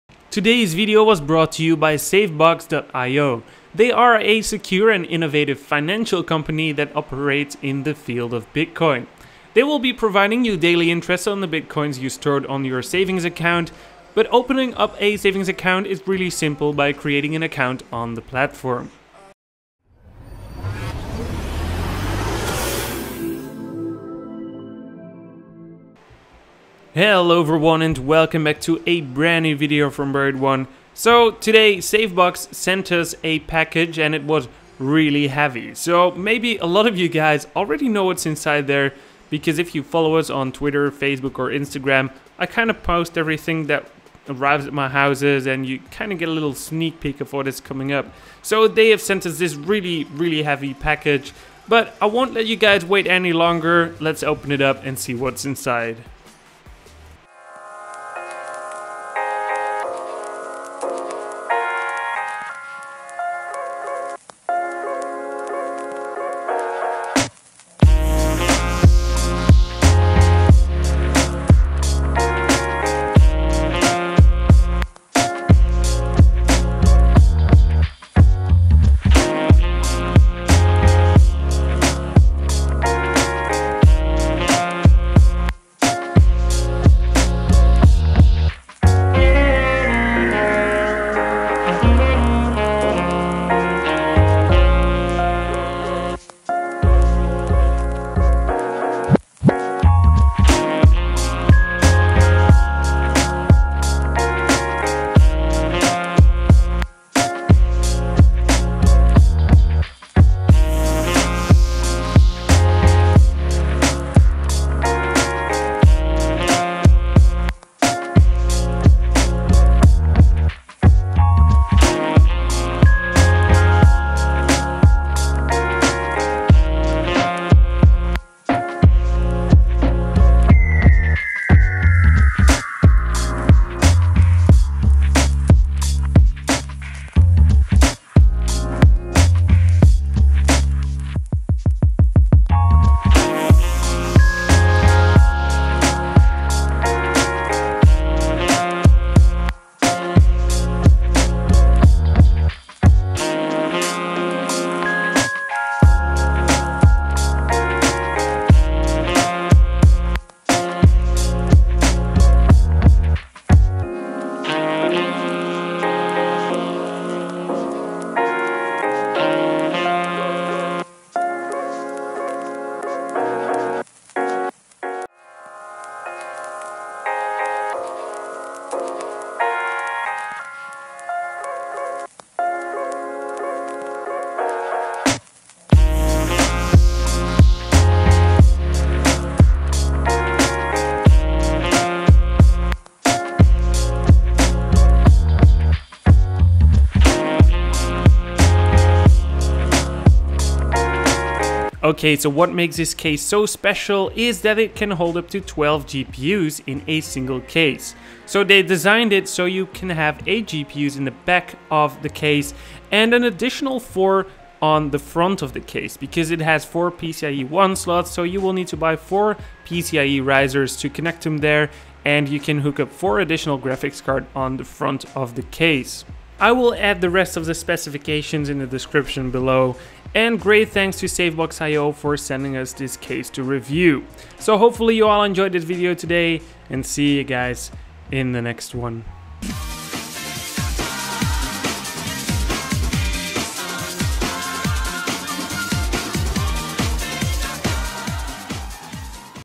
12-GPU Mining Case Sponsored by SaveBox
Today i show you a 12-GPU Mining case that was sent to us
The case has a serious potential for heavy load graphics cards, providing A solid 3300 Watts of Dual Mining Power Supplies.
It also includes a special designed motherboard to hold 8 Graphics cards straight on the board, and an aditional 4 with the use of PCIe Risers.
The moterboard is equiped with 4GB of Ram and a G4400 Intel CPU!

The case will be available for sale soon.